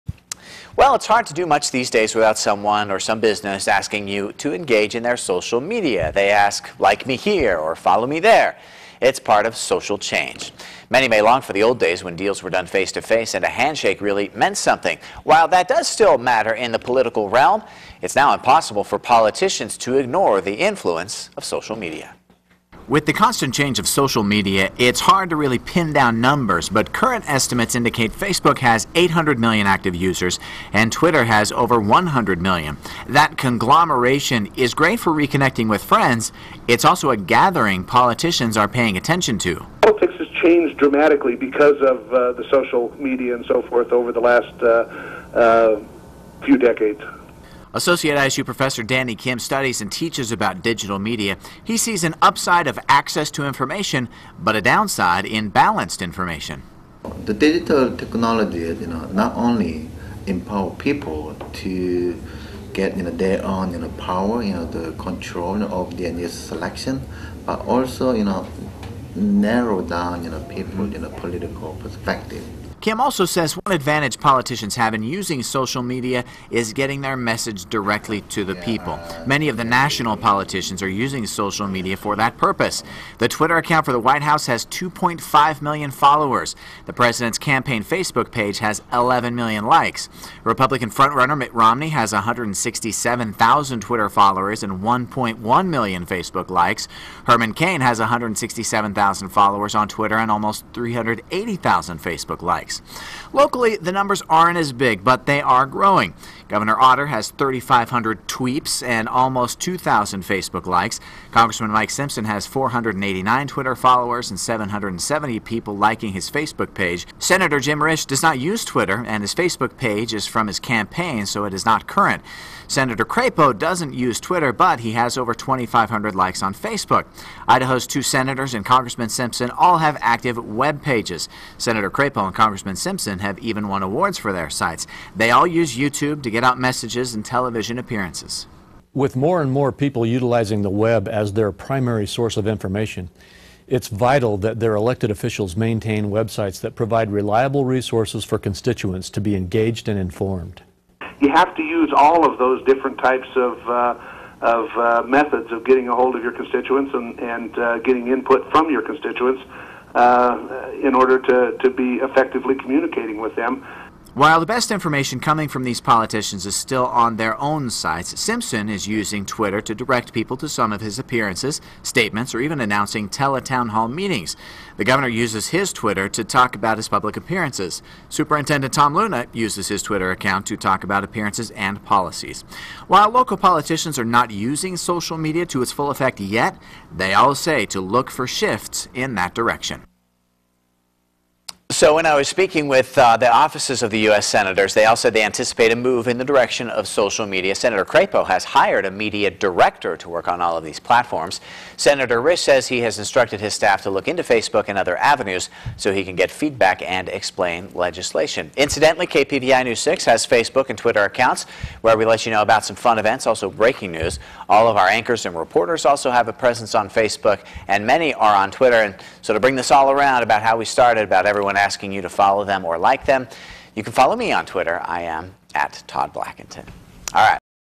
How Local Politicians Are Using Social Media
It's hard to do much these days without someone or some business asking you to engage in their social media.